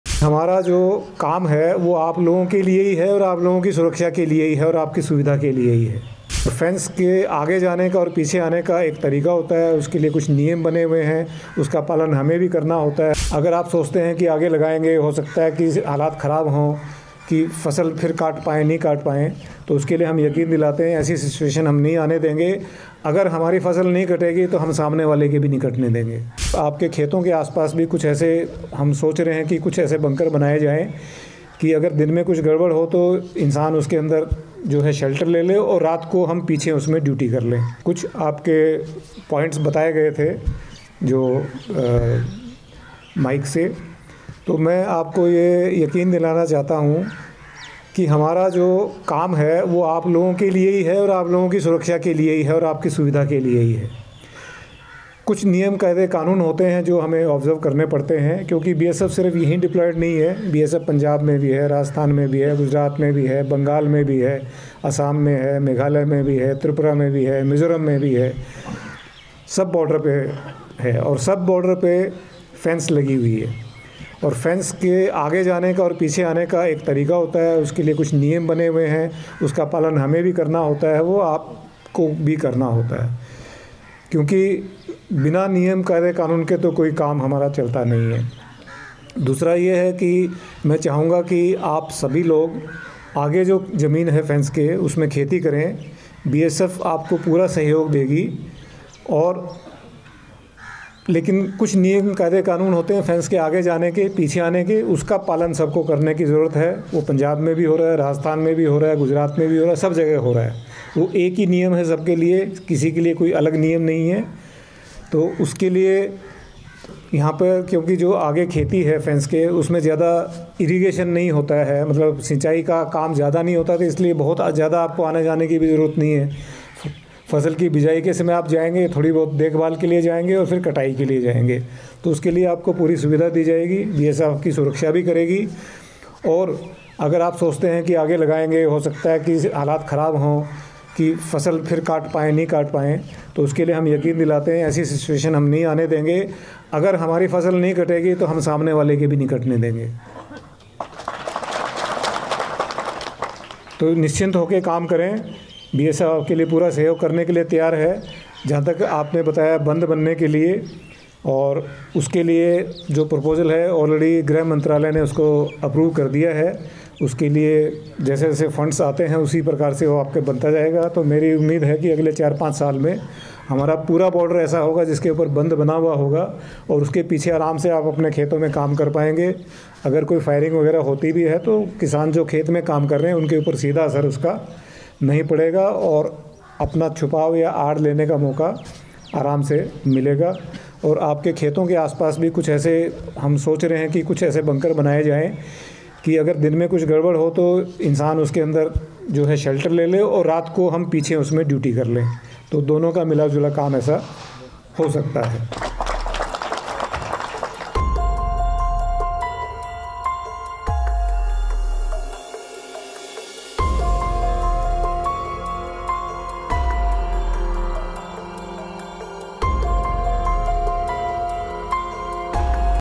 IG BSF’s direct message to Pakistan; assures complete safety of farmers on border areas
IG BSF DK Boora on February 29 reached Samba in Jammu and Kashmir. He listened to the grievances of the local farmers who live near the border. He assured them of their complete safety near the border areas and said that they will not face any issue in cultivation of their crops.